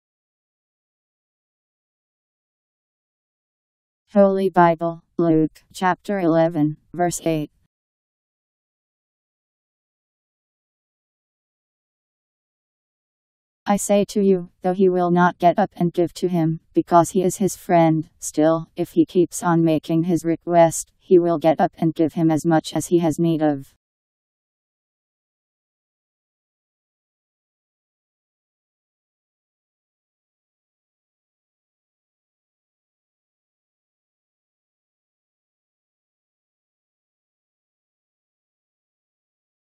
Holy Bible Luke 11:8
Video shows Holy Bible Luke Chapter 11 Verse 8: I say to you, Though he will not get up and give to him, because he is his friend, still, if he keeps on making his request, he will get up and give him as much as he has need of. Powered by MaryTTS. Text taken from Bible in Basic Edition. Bible Luke 11:8 Bible Learning, what Bible verse says.